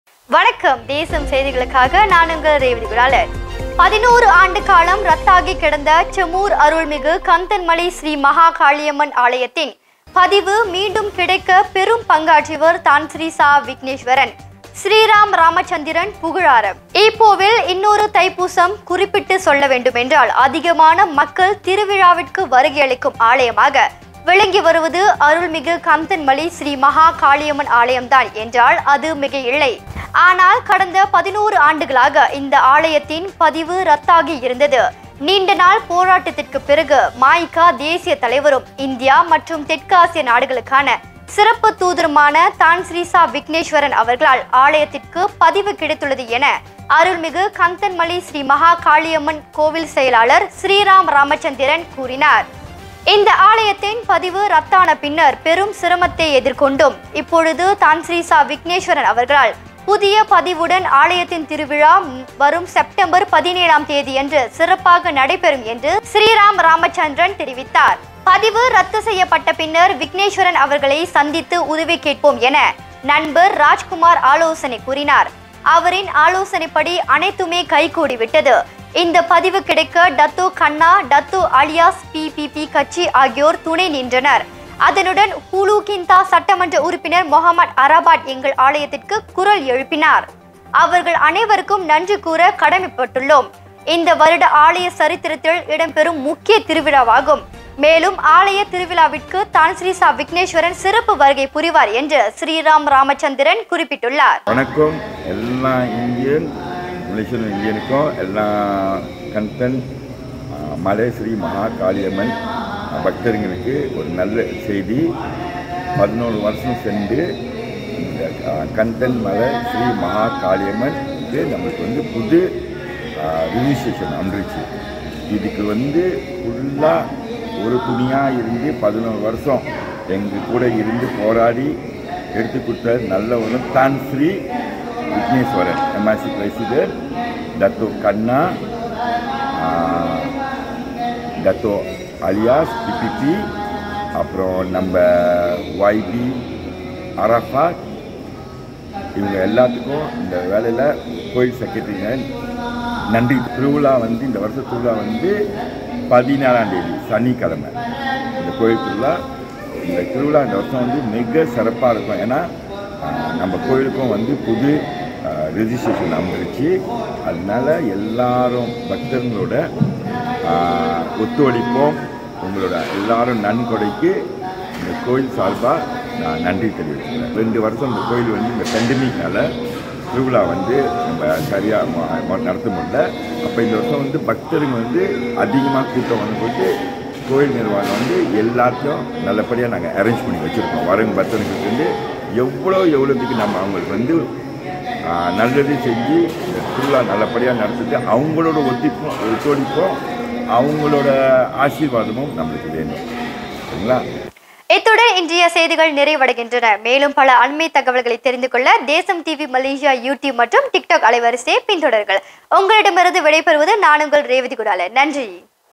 தான்ஸ்ரீ ச.விக்னேஸ்வரன் முயற்சியில் சிம்மோர் கந்தன் மலை ஸ்ரீ மகாகாளியம்மன் ஆலயத்தின் பதிவு கிடைத்தது
தான்ஸ்ரீ ச.விக்னேஸ்வரன் முயற்சியில் சிம்மோர் அருள்மிகு கந்தன் மலை ஸ்ரீ மகா காளியம்மன் ஆலயத்தின் பதிவு மீண்டும் கிடைத்தது